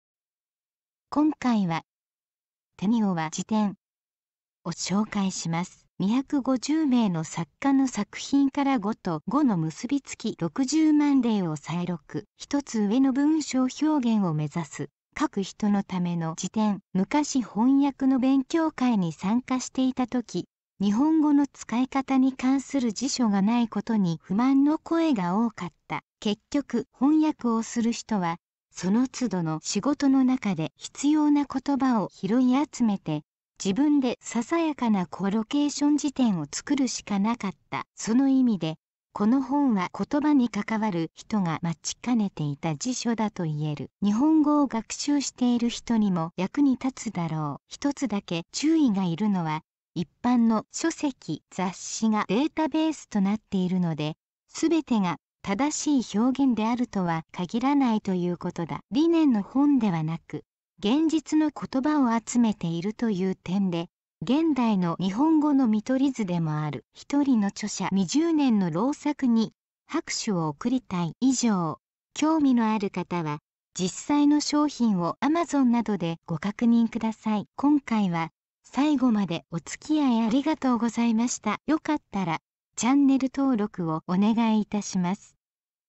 【紹介】てにをは辞典 （小内 一）
今回は、　てにをは辞典　を紹介します。

250名の作家の作品から語と語の結びつき60万例を採録。ひとつ上の文章表現をめざす「書く人」のための辞典。

昔翻訳の勉強会に参加していたとき、日本語の使い方に関する辞書がないことに不満の声が多かった。
結局翻訳をする人は、その都度の仕事の中で必要な言葉を拾い集めて、自分でささやかなコロケーション辞典を作るしかなかった。
その意味で、この本は言葉にかかわる人が待ちかねていた辞書だといえる。日本語を学習している人にも役に立つだろう。
ひとつだけ注意がいるのは、一般の書籍・雑誌がデータベースとなっているので、すべてが「正しい」表現であるとは限らないということだ。理念の本ではなく、現実の言葉を集めているという点で、現代の日本語の見取り図でもある。一人の著者20年の労作に、拍手を贈りたい。